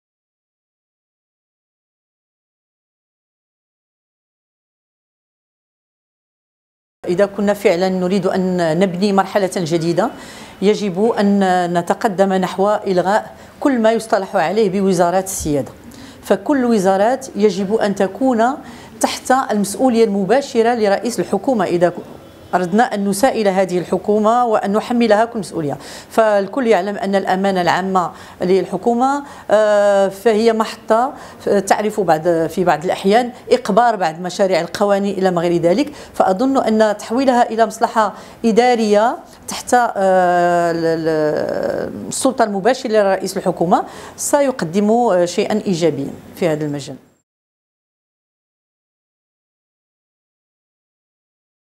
الأمانة العامة للحكومة ـ نبيلة منيب
"الأمانة العامة للحكومة تقبر أحيانا بعض القوانين" نبيلة منيب، الأمينة العامة للحزب الاشتراكي الموحد.